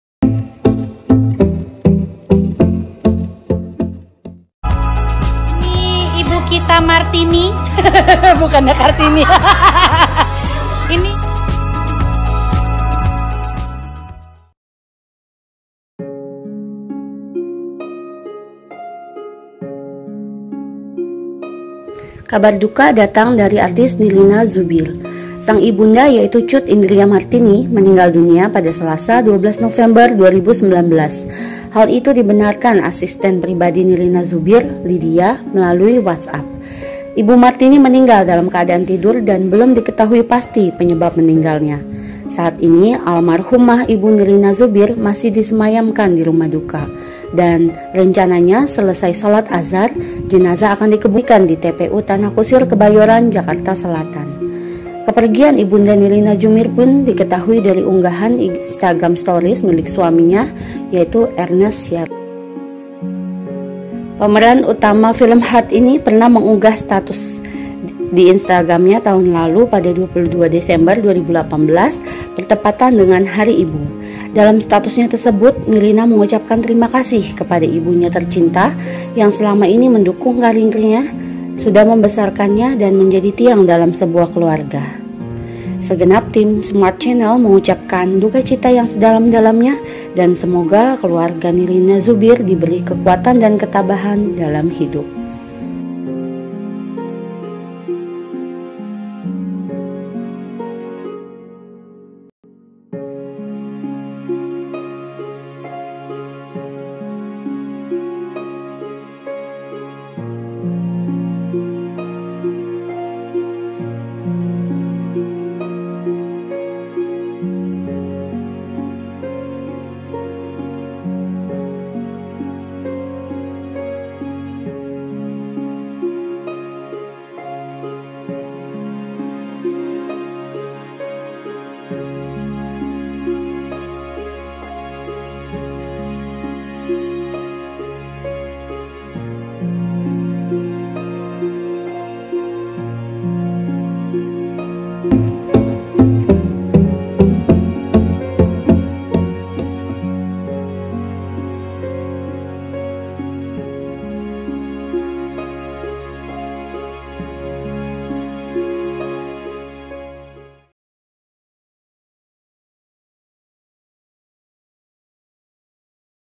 IBU NIRINA ZUBIR MENINGGAL DUNIA
Kabar duka datang dari artis Nirina Zubir... sang ibunda yaitu Cut Indria Martini meninggal dunia pada selasa, 12 november 2019.
Hal itu dibenarkan asisten pribadi Nirina Zubir, Lidia, melalui pesan WhatsApp.
Ibu martini meninggal dalam keadaan tidur dan belum di ketahui pasti penyebab meninggal nya.
Saat ini, almarhum ibu Nirina Zubir masih disemayakan di rumah duka. Dan Rencananya selesai salat Asar ...jenazah akan dikebumikan di TPU Tanah Kusir, Kebayoran, Jakarta Selatan.